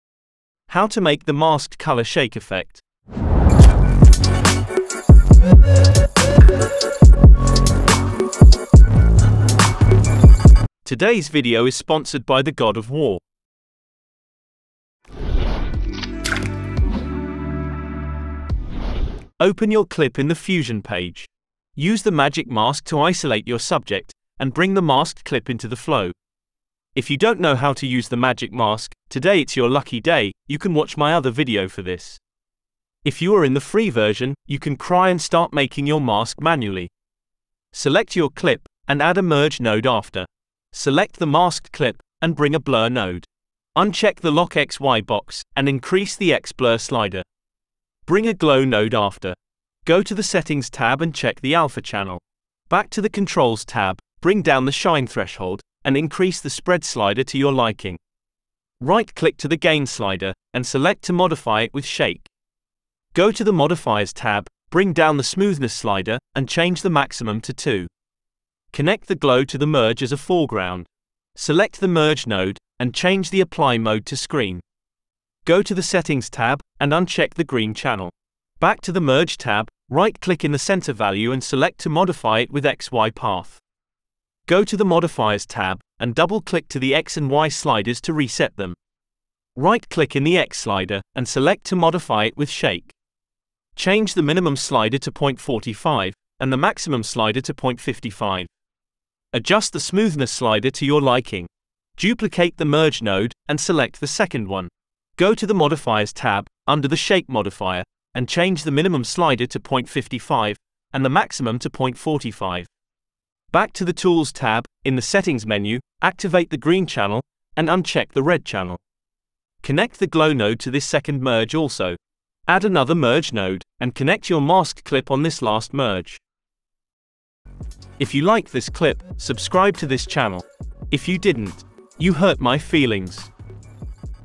Masked RGB Shake Effect | DaVinci Resolve 18 Studio
How to make the masked rgb shake effect in davinci resolve fusion page

you can send them here :

BTC : 1Ecg6F8UnFHnVxsKhbkecqtge11vBhqS5
ETH : 0xB643C1C8B6C75bcB3426F40f8514CB89Fc294abC